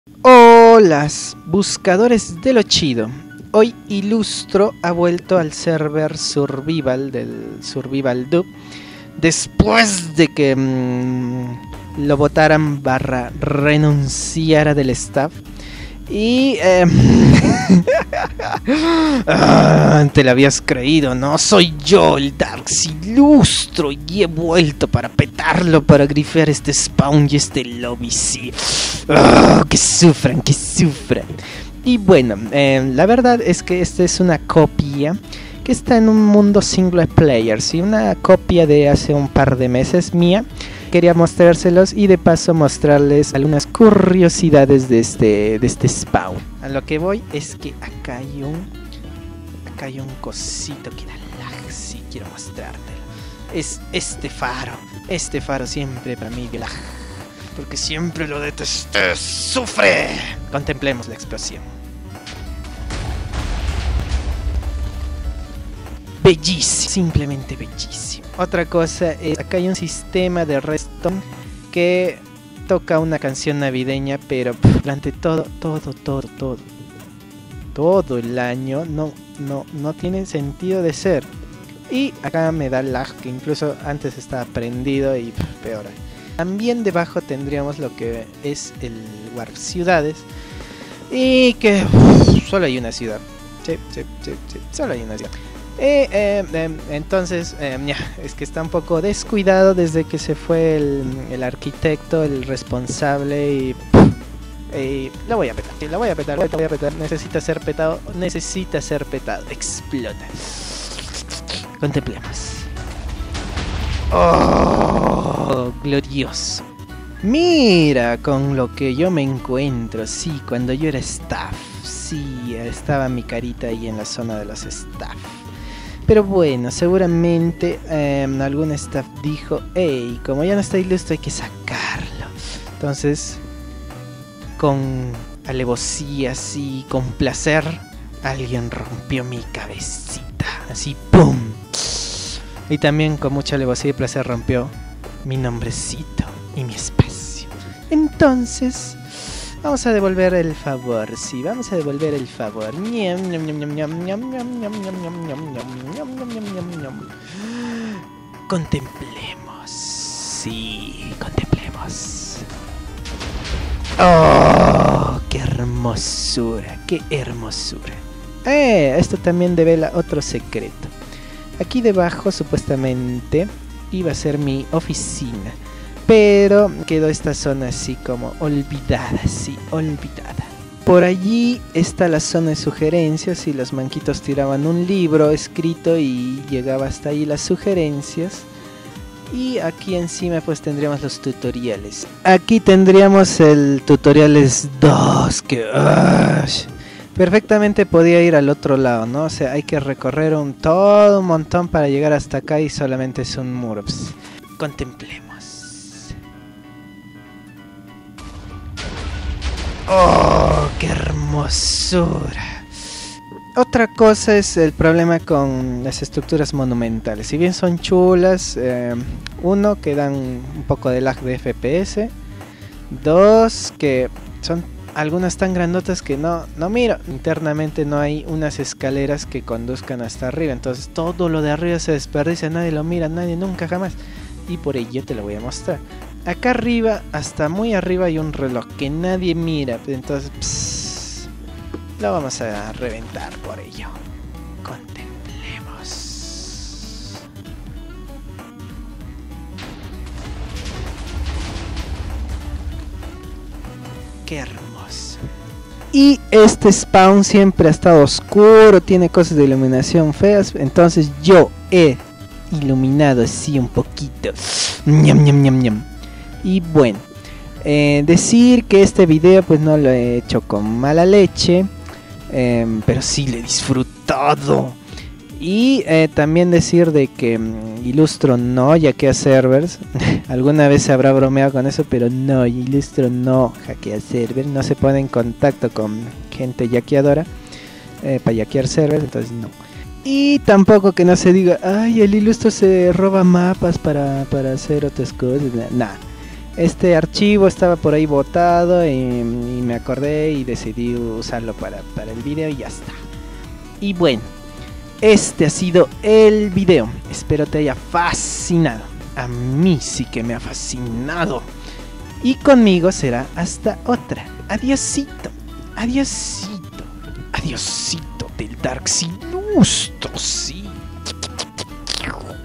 GRIFEO con TNT :} el Spawn SurvivalDub
-- Tu Like le da POWER al Darz Ilustro :} -- Hoy grifeamos una copia mía del spawn del survival del SurvivalDub. Es un mapa que tenía por allí botado en mi pc y quería grifearlo °-° , además de mostrar curiosidades sobre dicho Spawn
Canciones de fondo:
Pistas: grifeo grifing spawn lobby survival map mapa map dub curiosidades staff exstaff rango server admin explosión